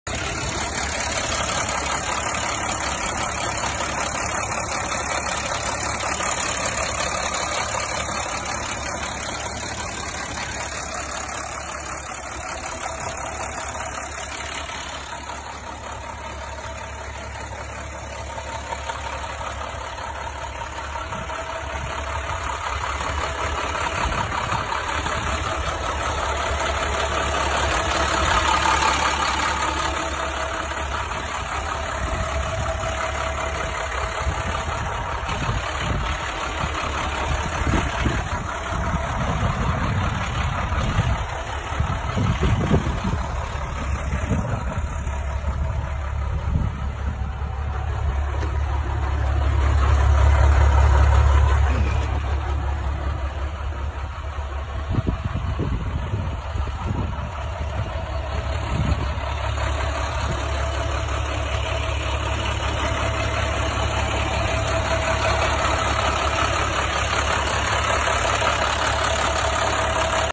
2012 VERSALIFT VN50I05 For Sale
good truck, runs and operates great, well maintained, air brakes, good tires, clean cab